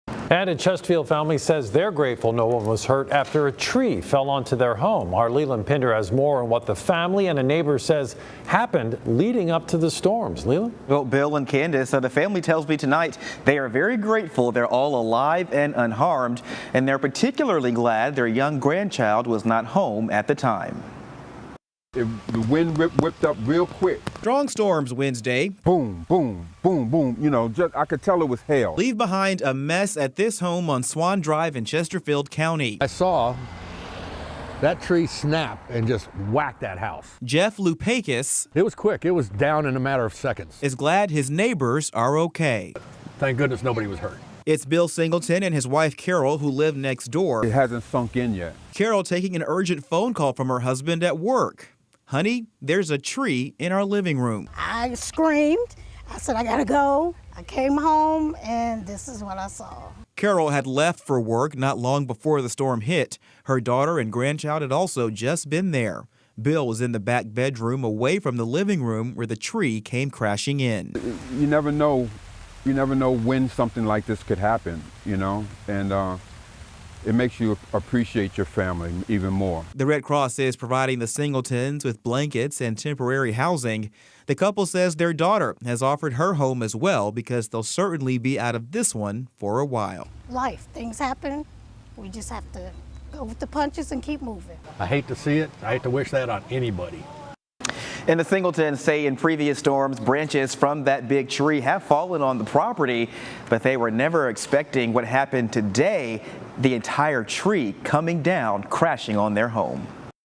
Tree crashes through Chesterfield family's home
Bill and Carole Singleton's home on Swan Drive fell victim to Wednesday's strong storms.